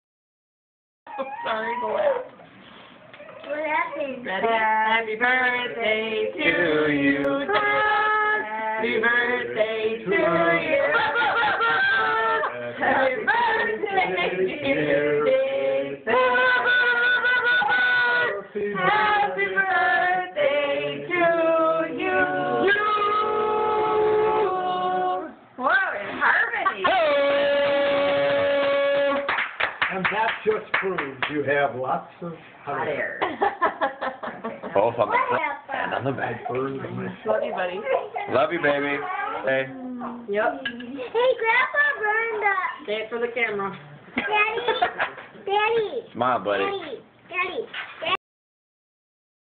Happy 13th Birthday Dathan
We celebrate Dathans 13th birthday, it is amazing that 13 years has gone by, I am truly blessed to have such wonderful children. Today we were able to celebrate my oldest, who continues to make me proud each and every day! My 1st born, love him with all my heart!